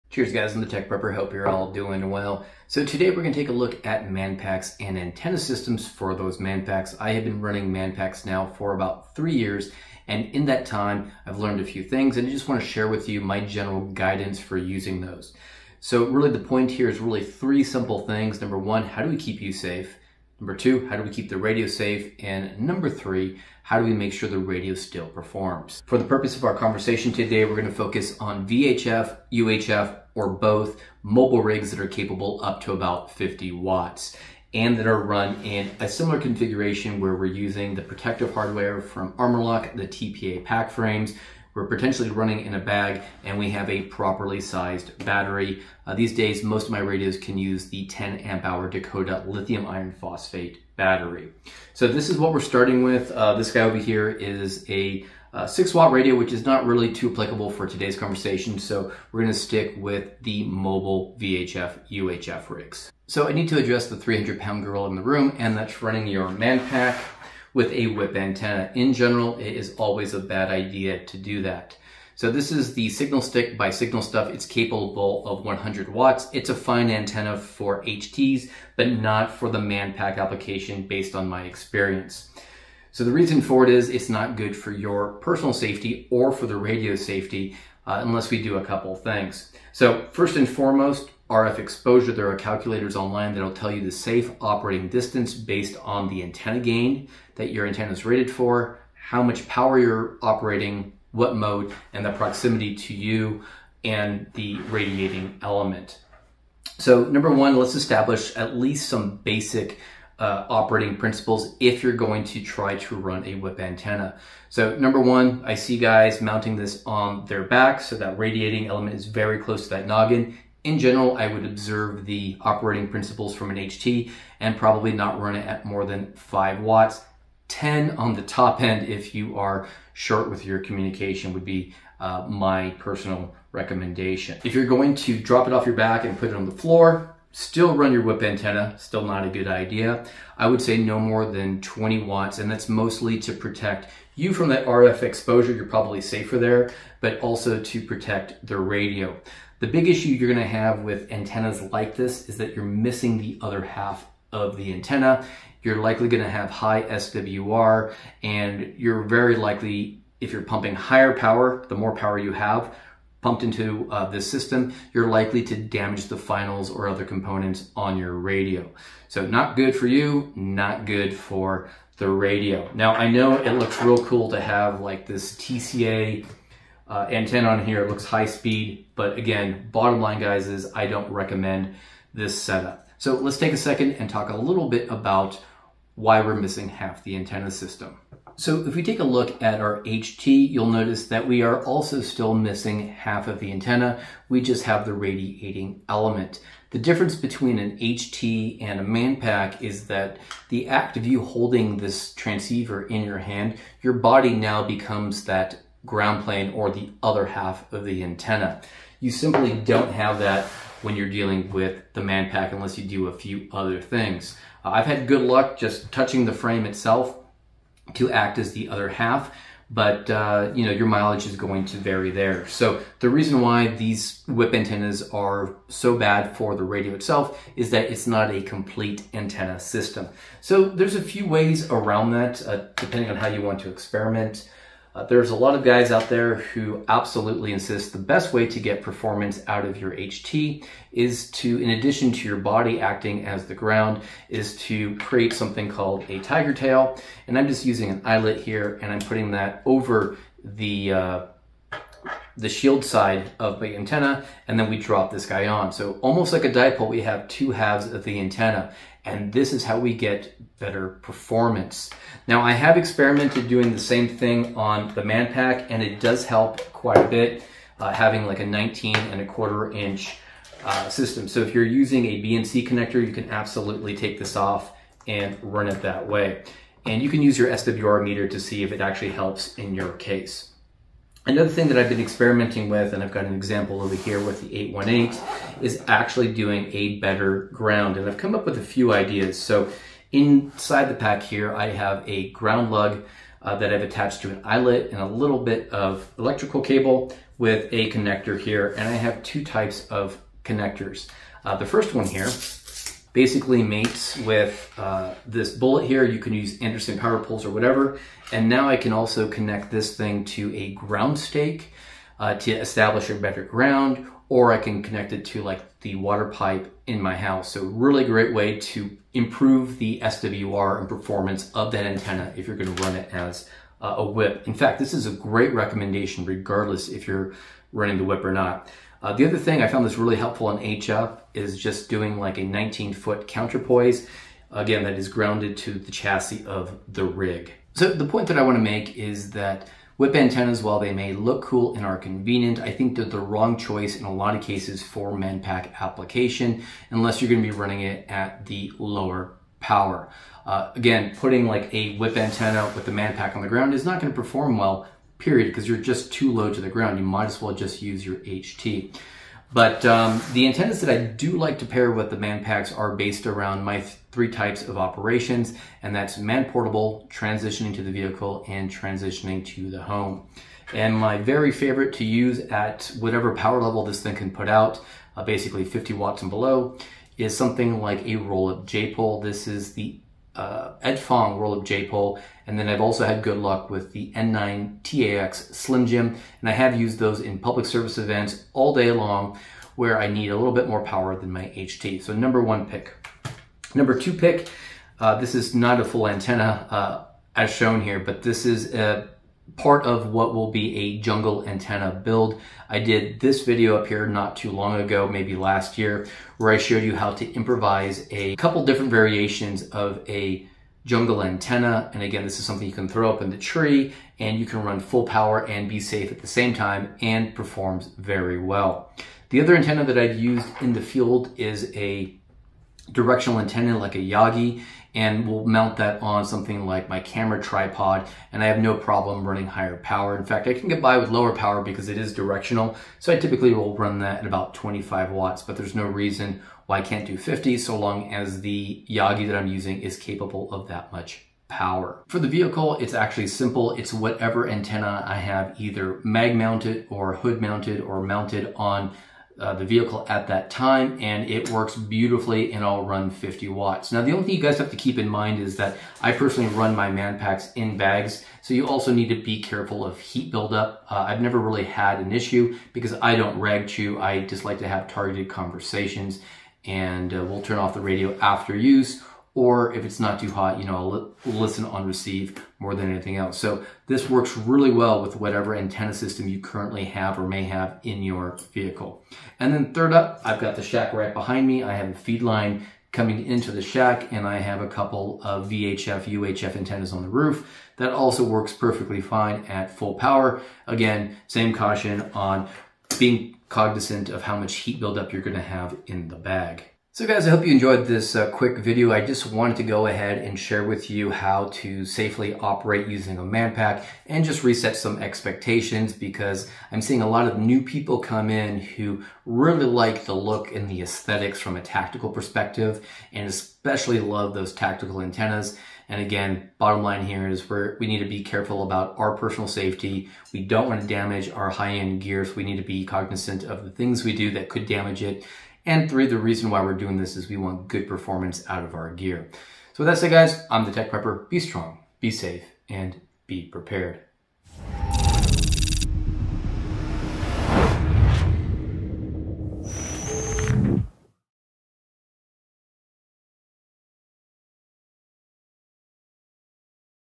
Manpack Radio Antenna Systems - Be Careful!
Are you interested in building a manpack radio? Here's what you need to know about your safety, protecting your radio, and getting good performance. In this video, I'll share some tips on selecting the right antenna.

JUNGLE ANTENNA
- Cobra Head
- 20 AWG Wire
- Ring Terminals
- S-clip Carabiners

0:00 Introduction
1:02 Whip Antennas
3:08 Antenna Performance Tips
5:37 Antennas for Higher Power